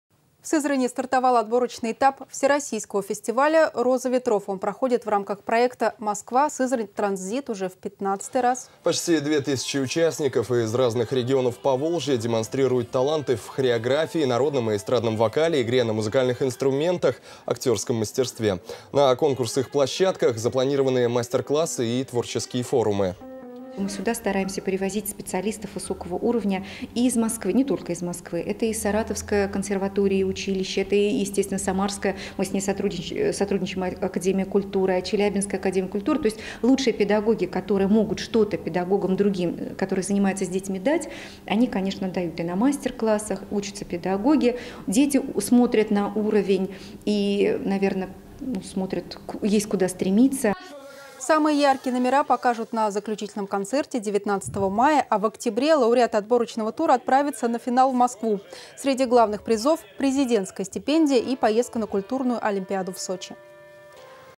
Всероссийский фестиваль "Роза ветров"
В Сызрани стартовал отборочный этап всероссийского фестиваля "Роза ветров". Он проходит в рамках проекта «Москва -- Сызрань транзит» уже в 15-й раз. Почти две тысячи участников из разных регионов Поволжья демонстрируют таланты в хореографии, народном и эстрадном вокале, игре на музыкальных инструментах, актерском мастерстве. На конкурсных площадках запланированы мастер-классы и творческие форумы.